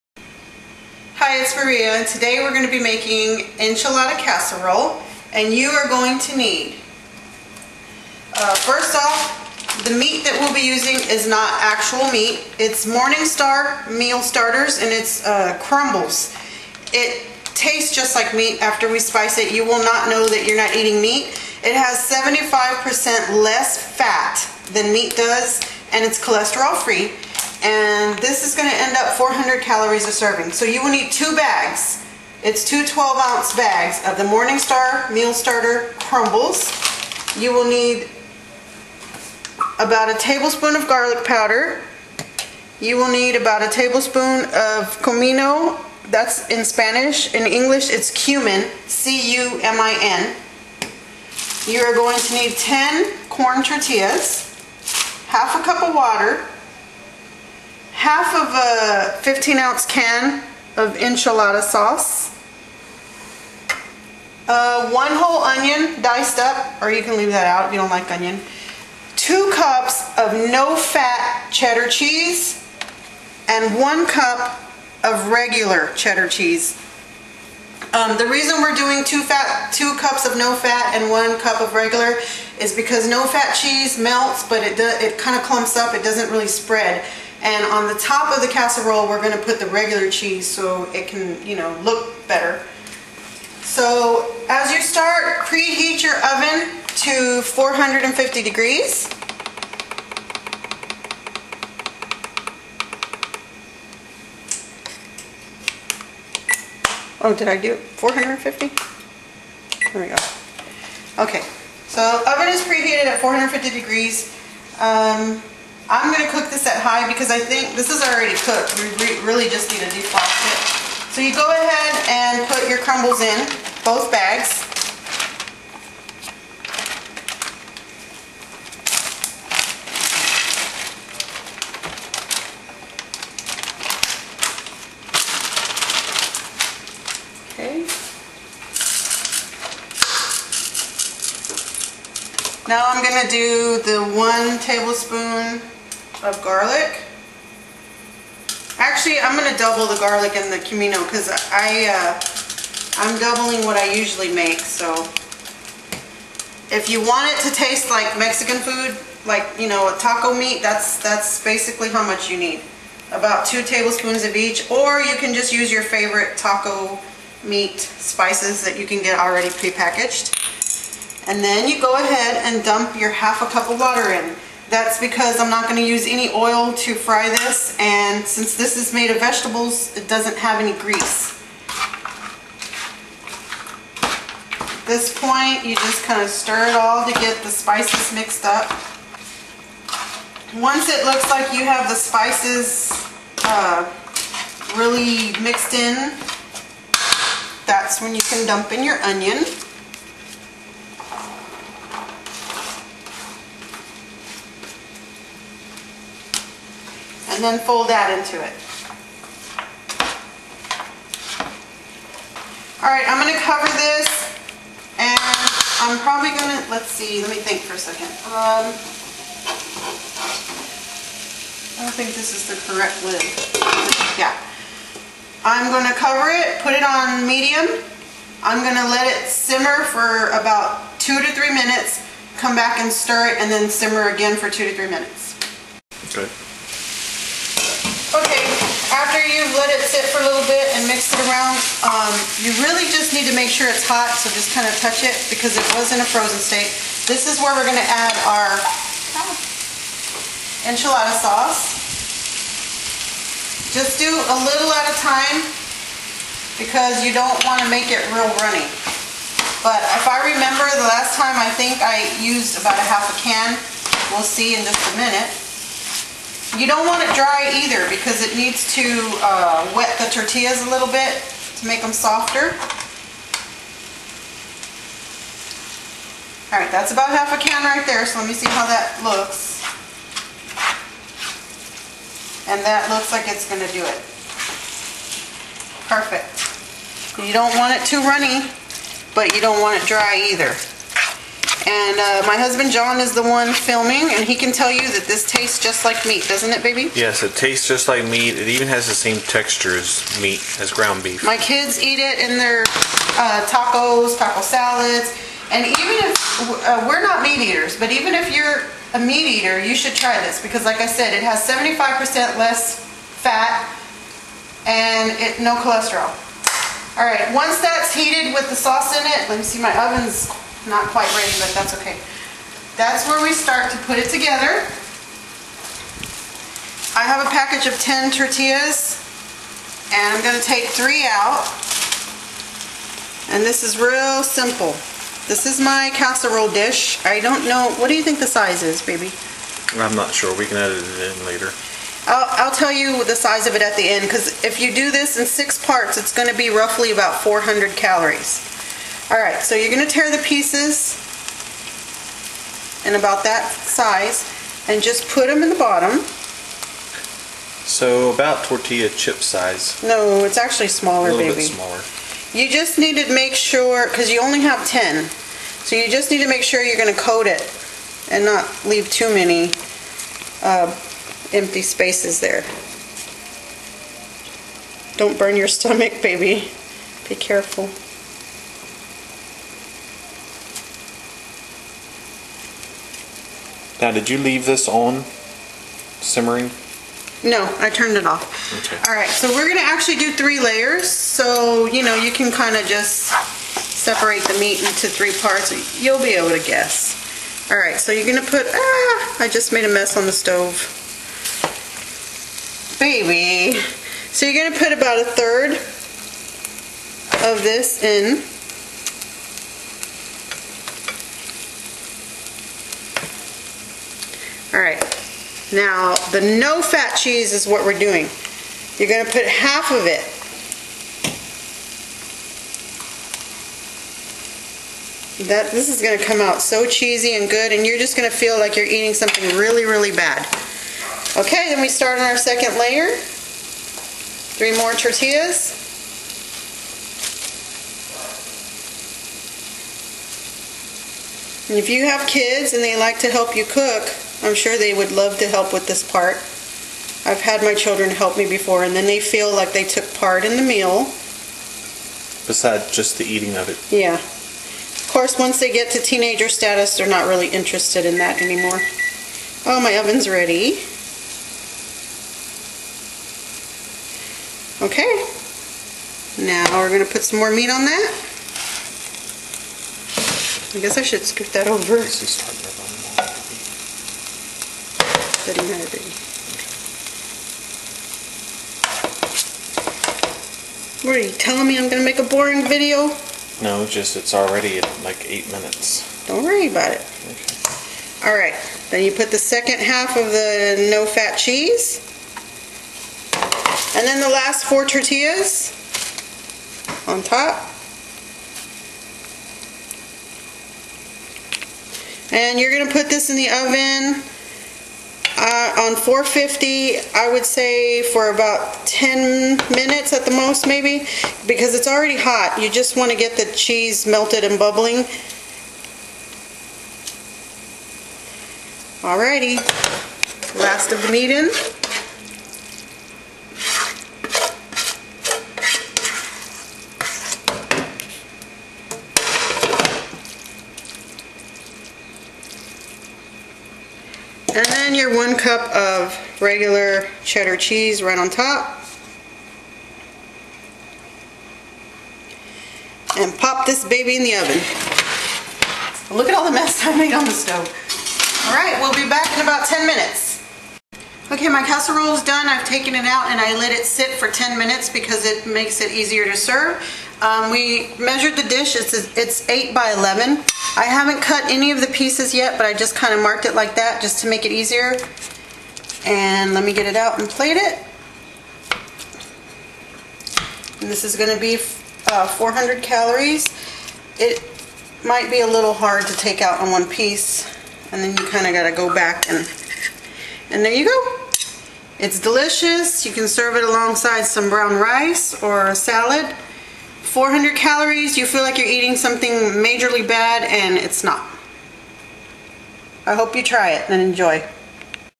Easy Enchilada Casserole (400 calories)
Enchilada Casserole

Ingredients
2 12oz bags of Morning Farms crumbles
2 tbsp garlic powder (not garlic salt)
2 tbsp cumin
1 whole medium onion
1/2 cup of water
1 can of 15oz enchilada sauce
2 cups no fat cheddar cheese
1 cup regular cheddar cheese
10 corn tortillas

Preheat oven to 450 degrees. Mix the 2 bags of crumbles, garlic powder, cumin and half a cup of water on high heat. Once mixed well, add the onion and mix again. Cover on a medium heat for 3 minutes and let it simmer. Stir then allow to simmer for about 3 minutes again. Add half a can of enchilada sauce and stir. Turn heat off and assemble the casserole in 3 layers. First layer is 3 of the corn tortillas torn in pieces, a third of the meat and 1 cup of no fat cheddar. Second layer is 3 more of the tortillas, a third of the meat and the other cup of no fat cheese. The last layer is the last 4 tortillas, the rest of the meat and then the 1 cup of regular shredded cheese. Pop this in the oven for 10 minutes at 450. Let sit for 10 minutes before serving. ENJOY!